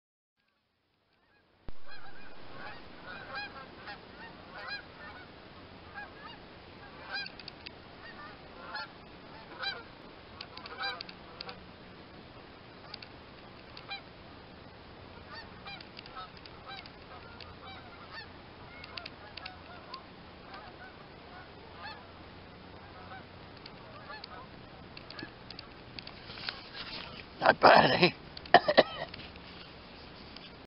Police Arrest Honking Geese In Ottawa!
Police have arrested Honking Geese In Ottawa at the Truckers Freedom Rally